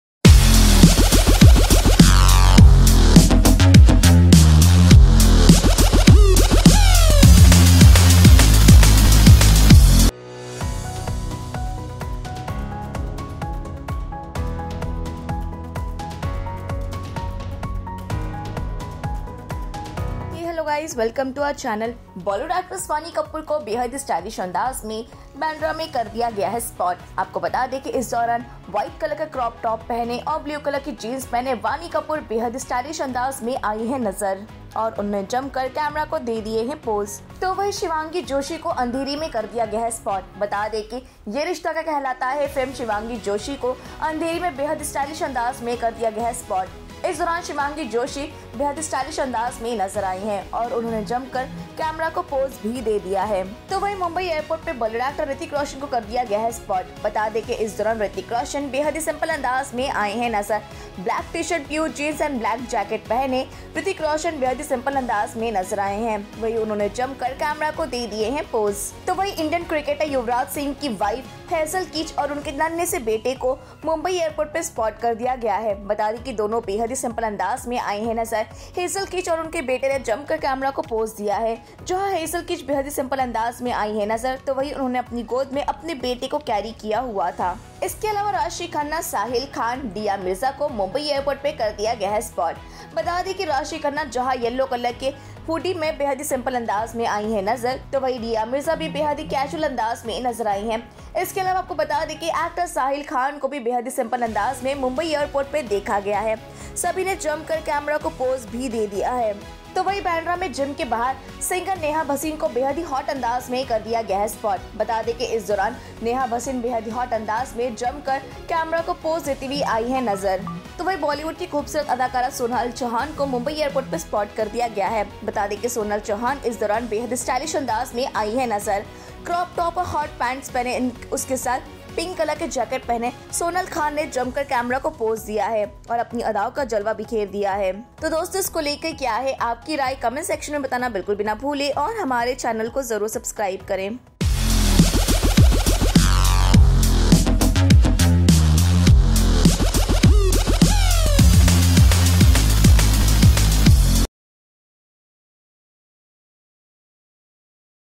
Hrithik, Sonal flaunt their airport looks | Bollywood News | ErosNow eBuzz
The celebs posed happily with their fans at the Mumbai airport